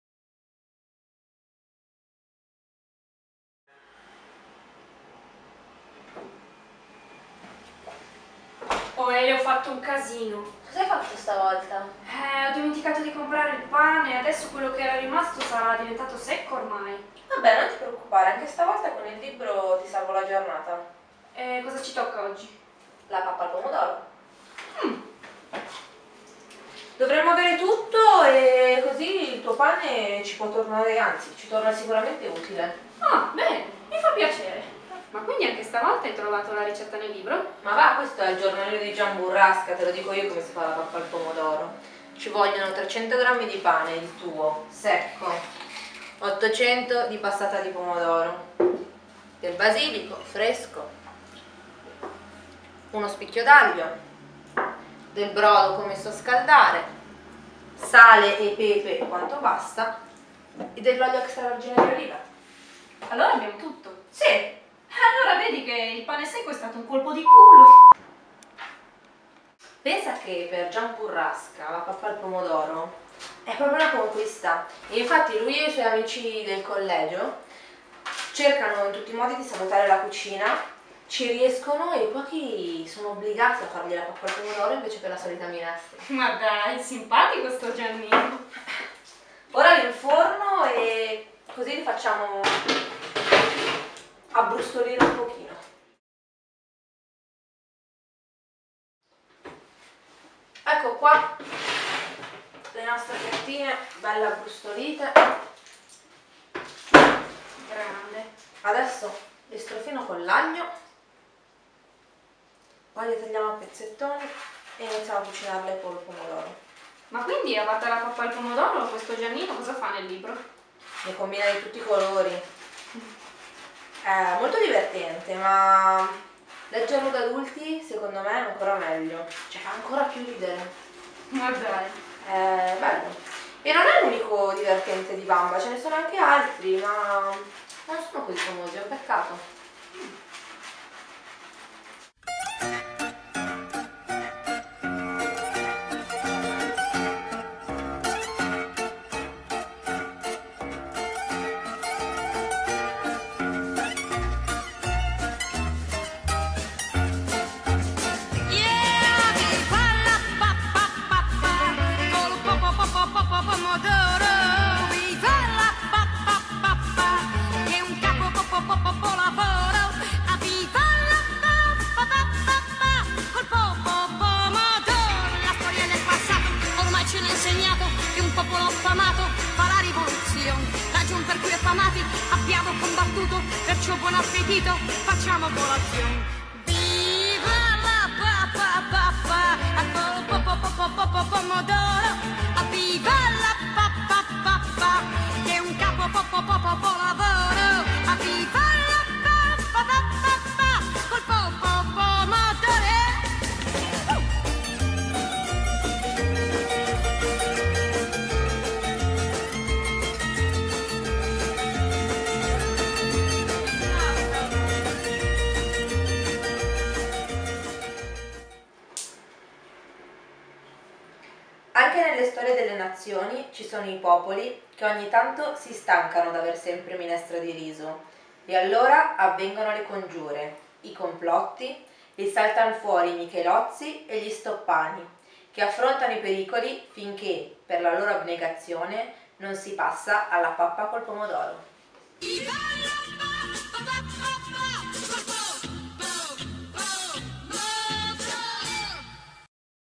Pappa al Pomodoro - "Il Giornalino di Gian Burrasca", Vamba.
Vi siete sbellicati dalle risate con le avventure di Giannino? E allora cimentatevi con la golosa ricetta della pappa al pomodoro.